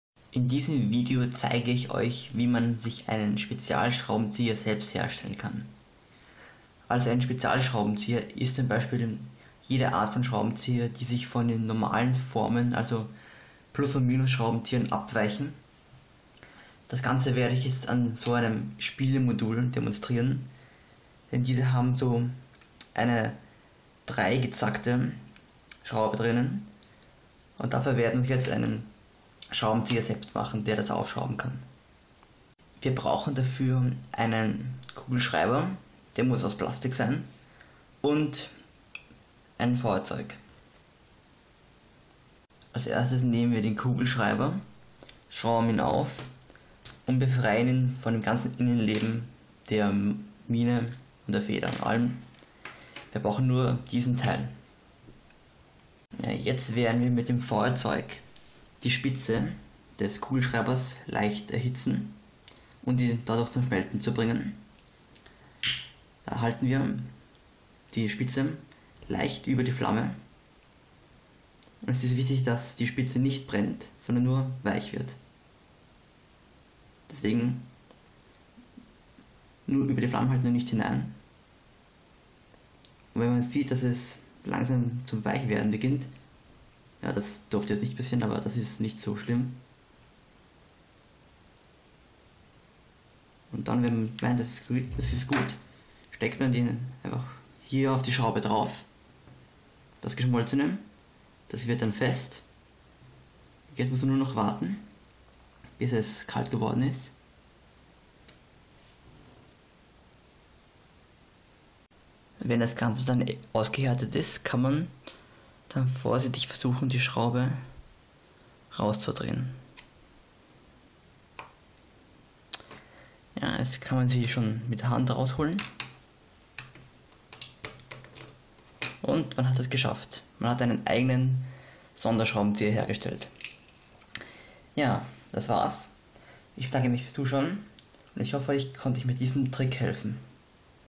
Spezial Schraubenzieher selber bauen - Tipp für Heimwerker
- Wenn ihr mal einen besonderen Schraubenzieher braucht, müsst ihr euch keinen teuren Spezialschraubenzieher kaufen, sondern ihr könnt ihn euch ganz einfach und schnell selber bauen. Alles was ihr dazu benötigt ist ein Kugelschreiber und ein Feuerzeug.

Wenn auch ihr mit hilfreichen Videos Geld verdienen wollt, dann schaut doch mal bei ClipFlip vorbei: